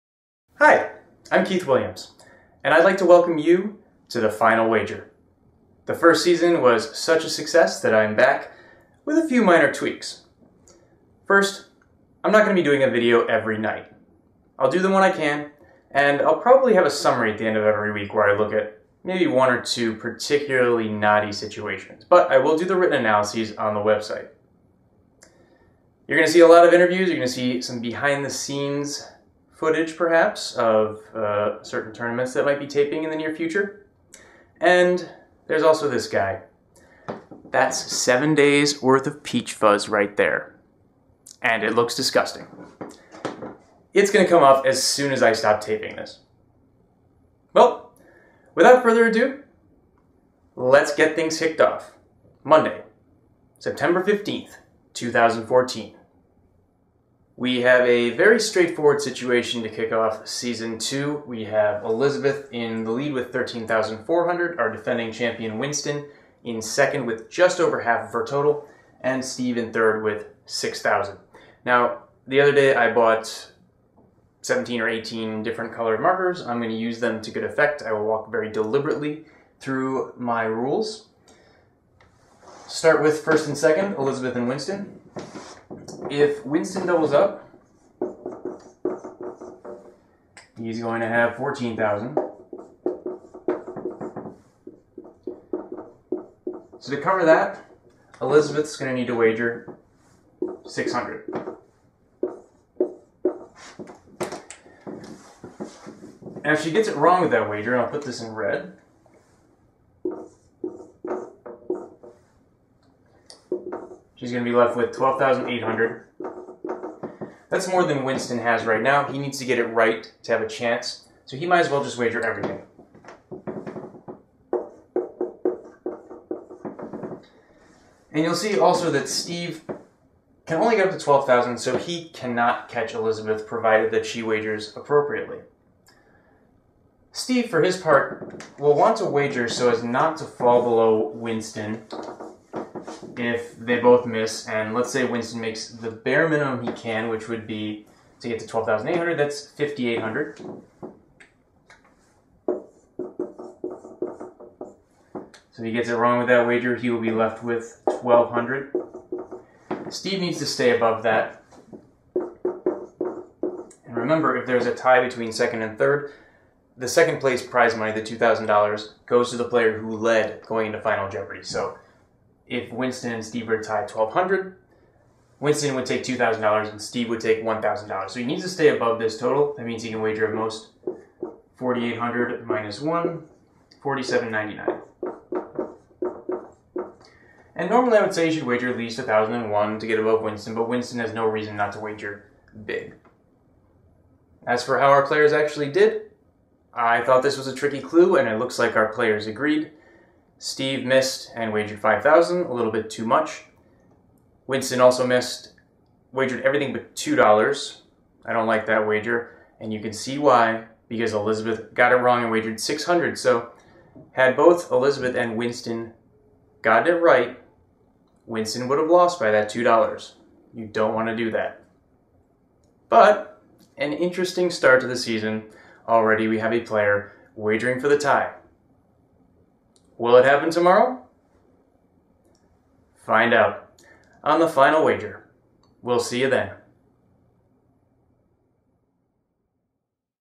The Final Wager – Monday, September 15, 2014
2003 Jeopardy! College Champion Keith Williams breaks down the wagering for the match Winston Nguyen vs. Elizabeth Williams vs. Steve Lozano, which first aired September 15, 2014.